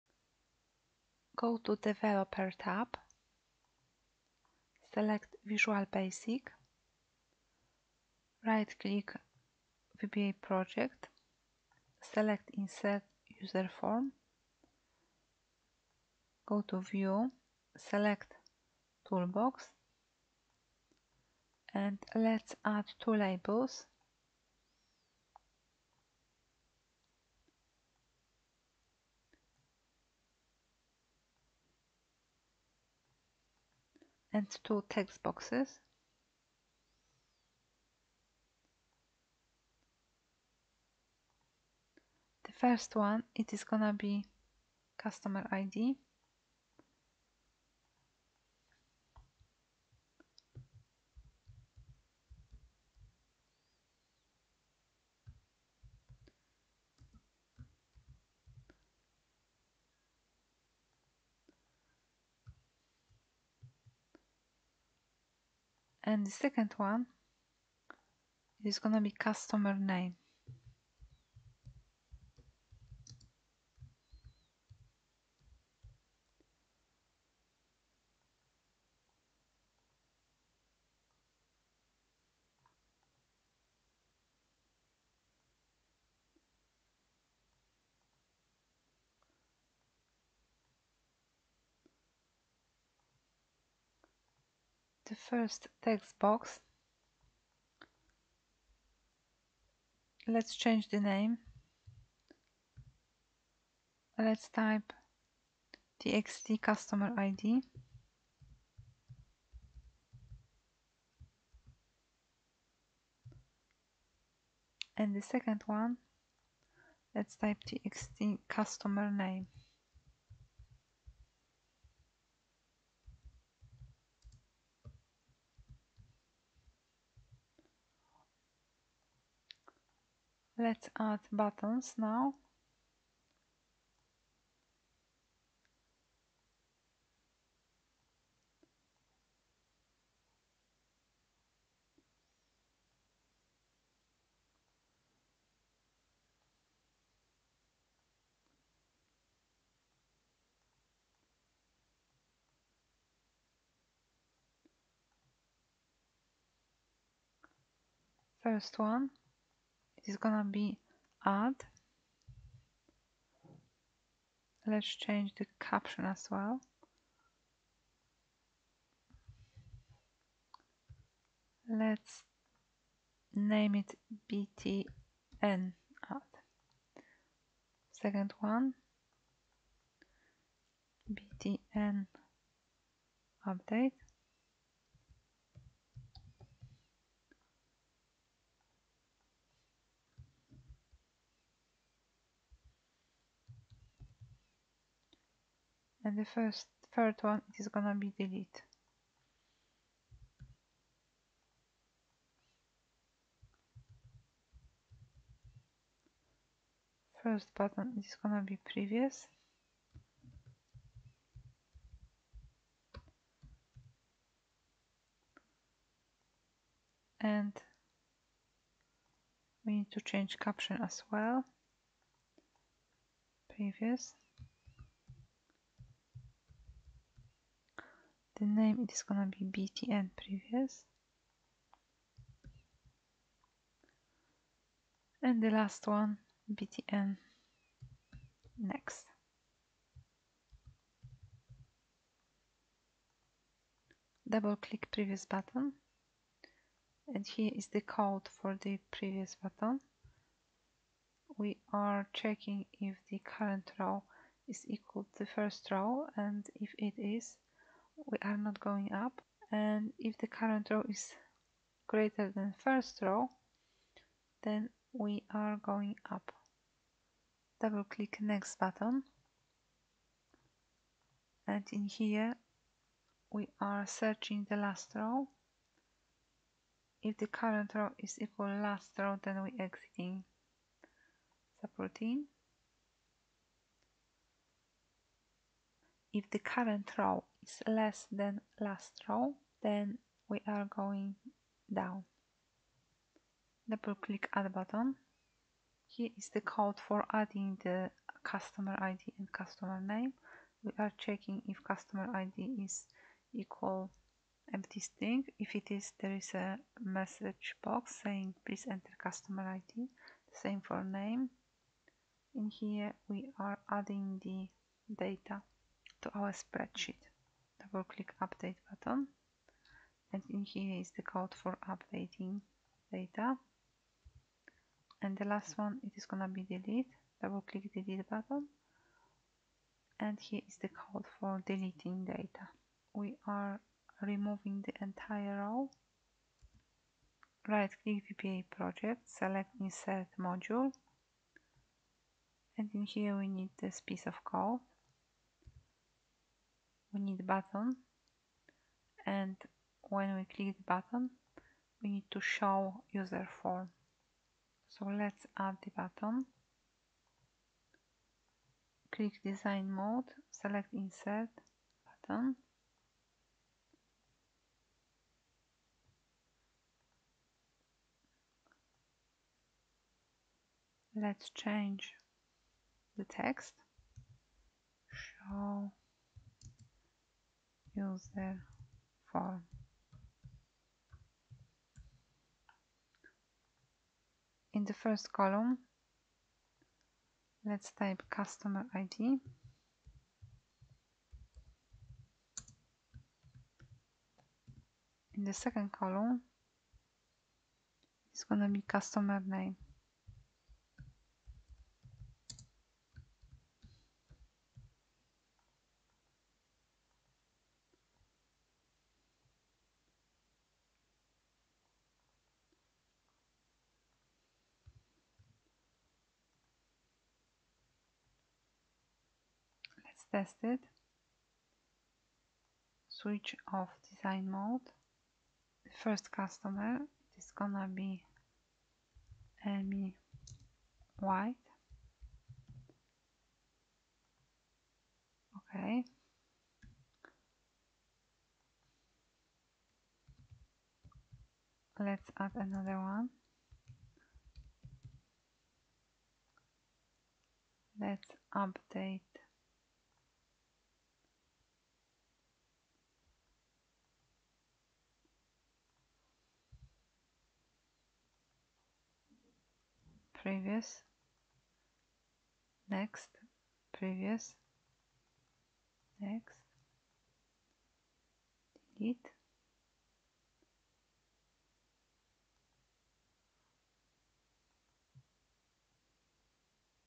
Excel VBa - How To Create An Excel Userform To Add Edit And Delete
Learn How To Create An Excel Userform To Add Edit And Delete